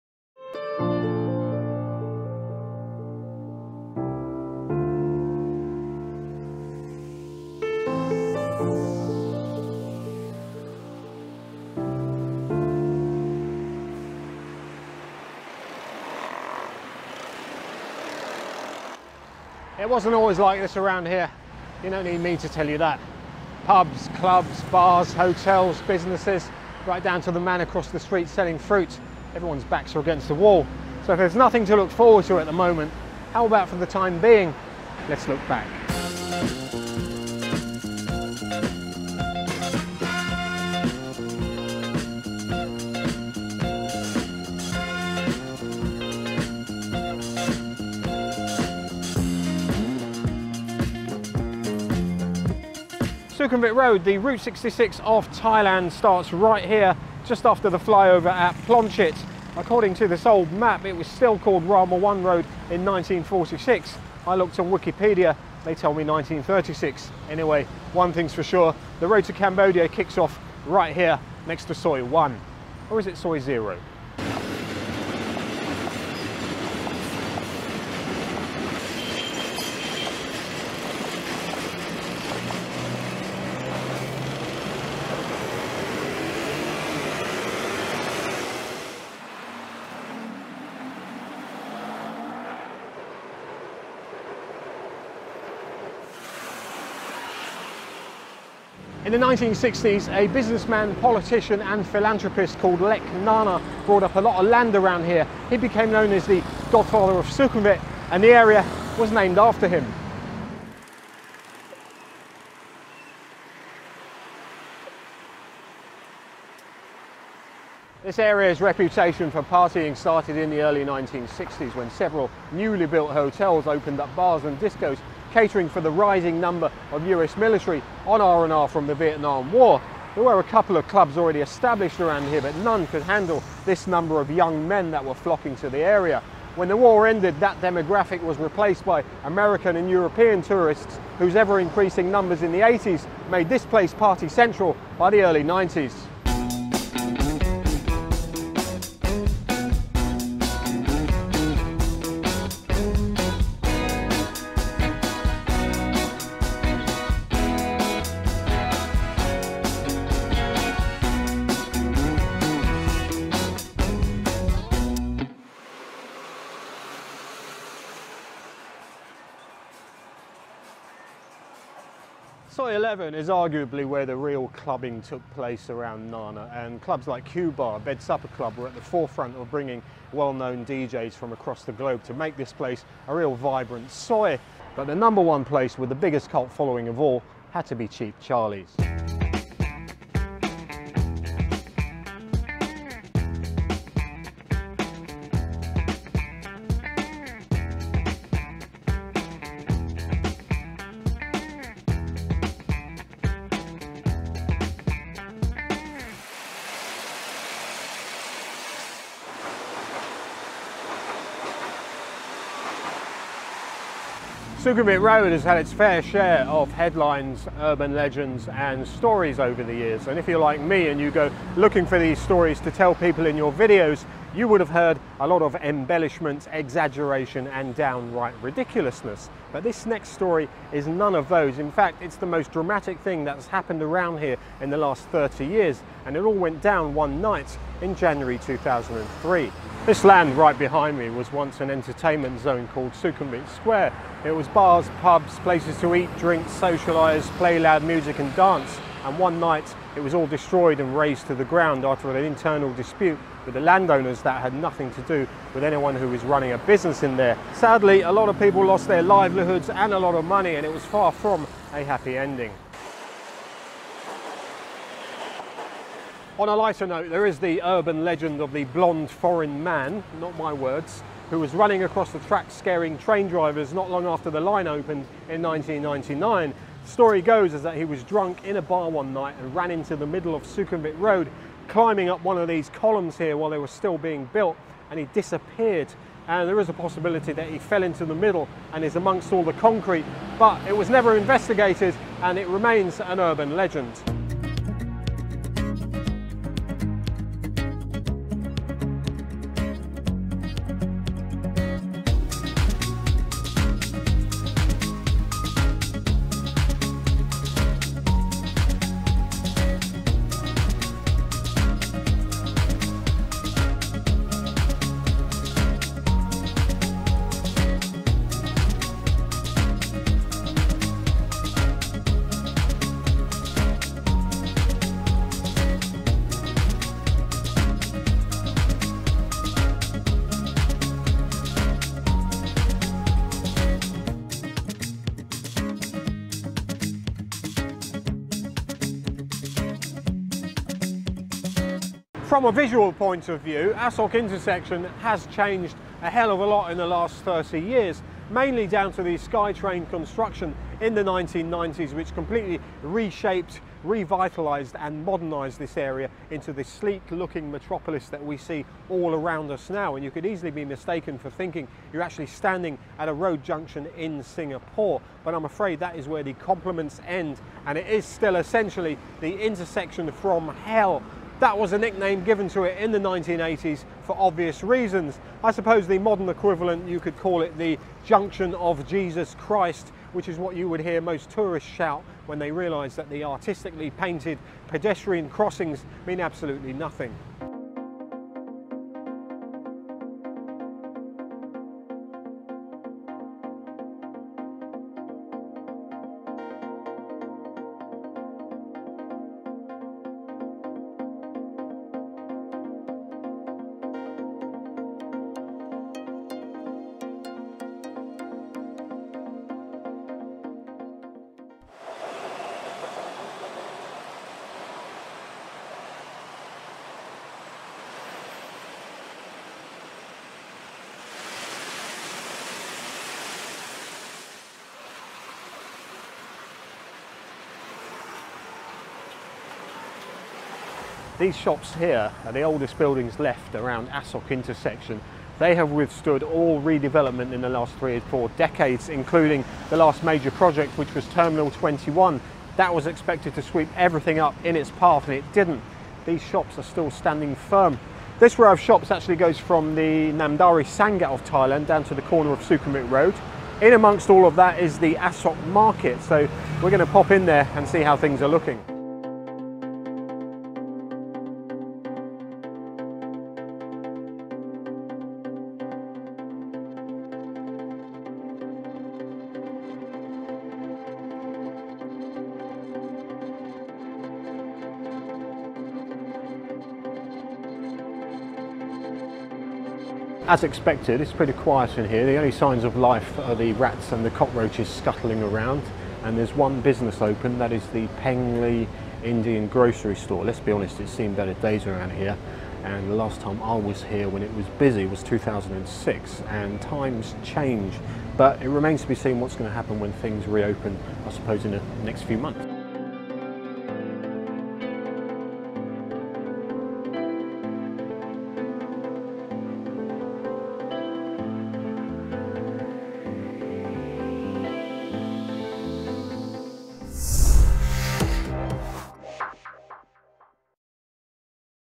Sukhumvit Road: A Brief history of Bangkok, Thailand - Part 1
With 'Bangkok Pat'. Sukhumvit Road: Let's look back. The future is uncertain, we can't look forward, so let's look back for 10 minutes. Nana to Asok is probably the most famous part of Sukhumvit Road, and it's changed a lot in the last few decades. From it's beginnings 50yrs ago and the history of its name, we look at how it's changed, the streets, the traffic, the skytrain, the buildings, the nightlife, as well as a few headlines and urban legends.

More to come on The Thaiger in this special series with Bangkok Pat.